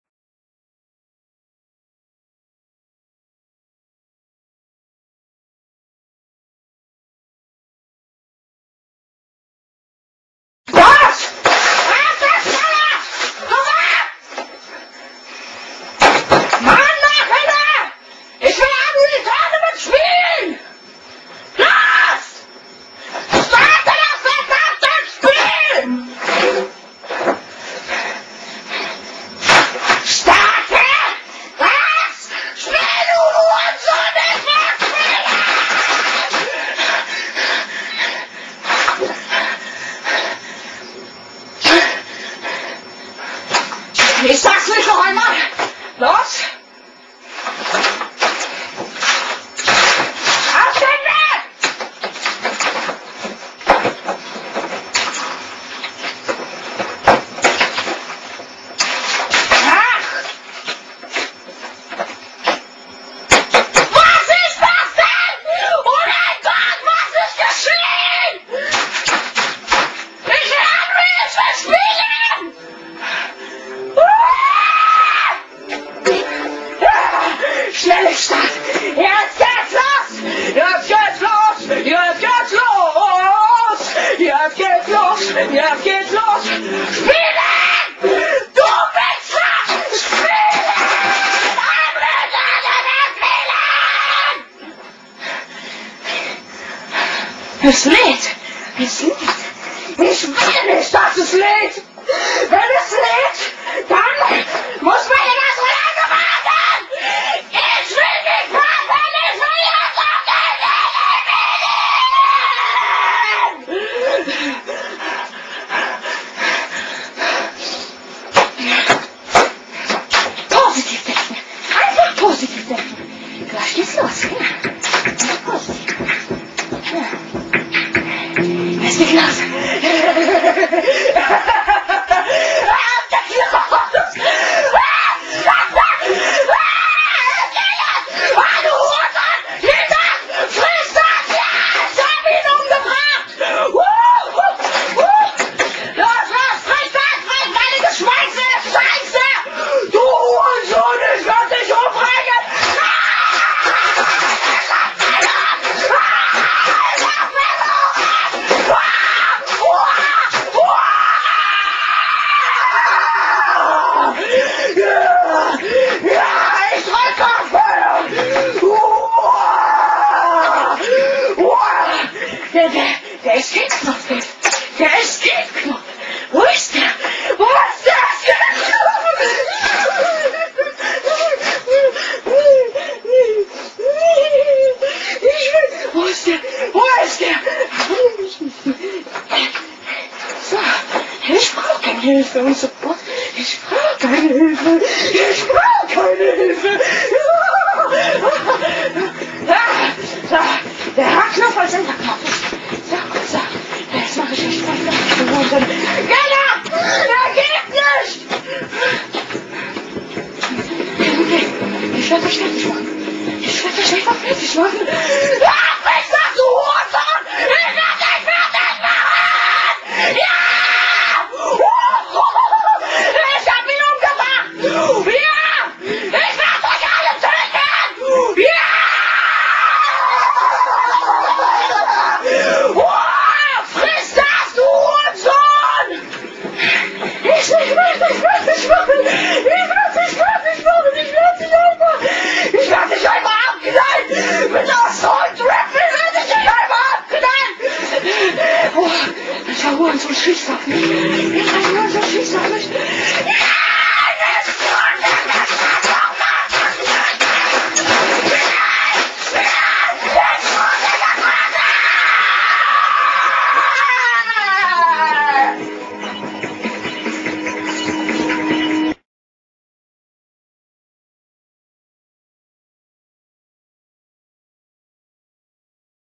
Angry German Kid - Correct English Translation + Subtitles
After seeing so many stupid (or rather wrong) translations of what the Angry German Kid is saying, I felt it has to be someone's duty to translate it and add some subtitles. Enjoy!